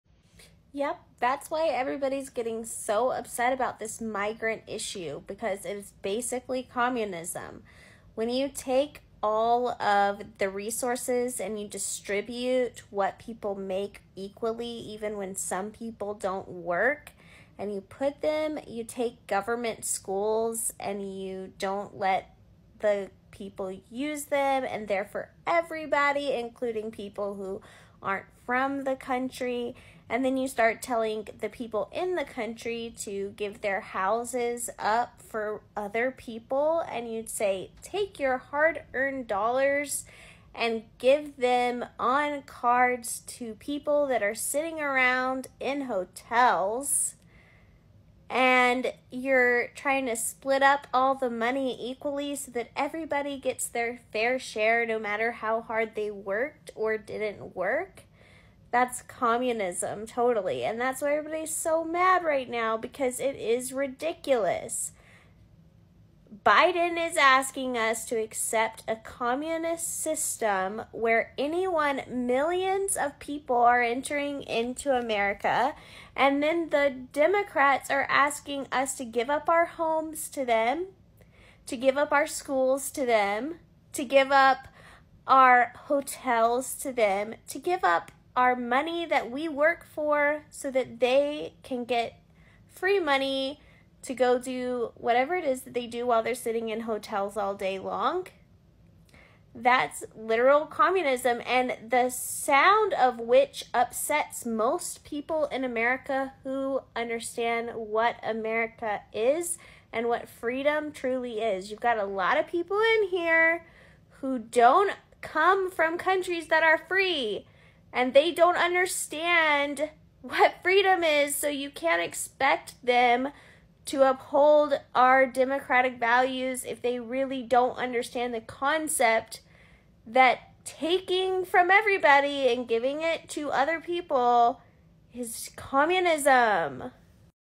This Border Crisis Has Led America to Communism (Airports, Schools, Pay Cards and American Homes)
Communism is a type of government as well as an economic system (a way of creating and sharing wealth). In a Communist system, individual people do not own land, factories, or machinery. Instead, the government or the whole community owns these things. Everyone is supposed to share the wealth that they create.

In America we are forcing citizens to pay for millions of migrants to be housed in Government funded facilities, airports, public schools and now they are asking us to give up our homes for migrants. In addition they are redistributing our wealth to migrants further adding to the communism sweeing our country. what is communism? its the redistribution of wealth so that non working people will be taken care of by the working. our founding fathers believed we should not be burdened by taxes and leaders who lord over the people.

this is against the founding fathers dreams for this country and not at all what this nation was founded on.

We have fought for years to be a shining light of democracy, but now we are spitting in the faces of our forefathers, by allowing our goverment leaders to forget every value we hold dear.